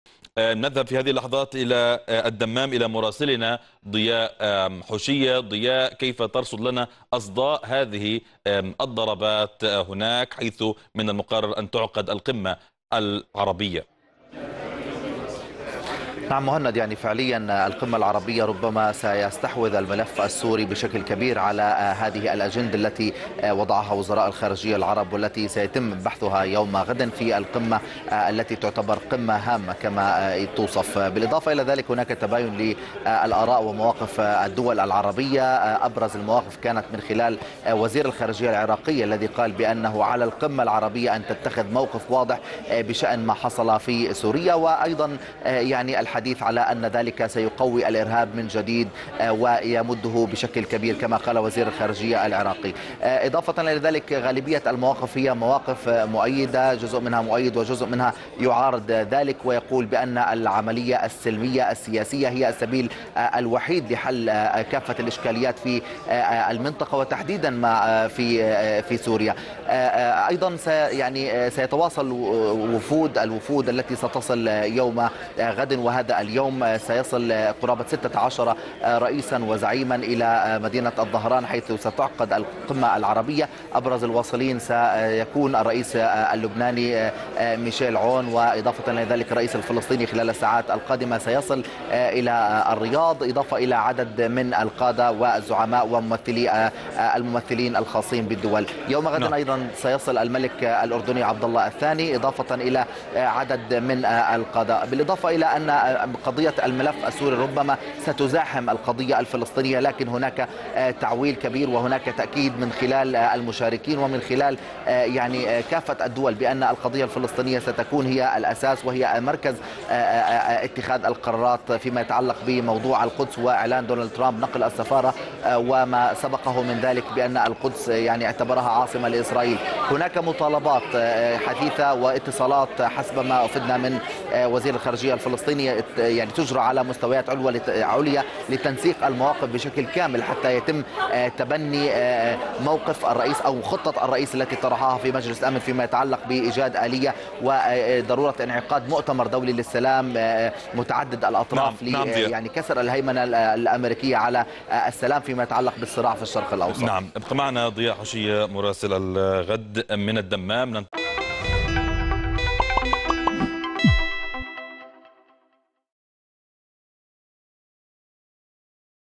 موفد الغد: 16 رئيسا سيصلون مدينة الظهران للمشاركة في القمة العربية وتباين في وجهات النظر حول سوريا
قال موفد قناة الغد إلى الدمام، ضياء حوشية، إن الملف السوري سوف يستحوز على أجندة أعمال القمة العربية في الدمام السعودية التي وضعها وزراء خارجية العرب التي سيتم بحثها يوم غد، فهناك تباين في الآراء وفي مواقف الدول العربية أبرزها كانت من خلال وزير الخارجية العراقي الذي أكد أنه على القمة العربية أن تتخذ موقف واضح بشأن ما حصل في سوريا على أساس أن هذا سيقوي الإرهاب من جديد ويمده بشكل كبير، باقي المواقف جزء منها مؤيد للضربة الأمريكية في سوريا وجزء أخر يعارض لافتين إلى أن العملية السلمية السياسية هي السبيل الوحيد لحل كافة الإشكاليات في المنطقة، مؤكدا أنه سيصل قرابة 16 رئيسا وزعيما إلى مدينة الظهران السعودية حيث تعقد القمة العربية.. التفاصيل يسردها خلال مشاركته عبر النشرة الإخبارية، مع الإعلامي مهند العراوي.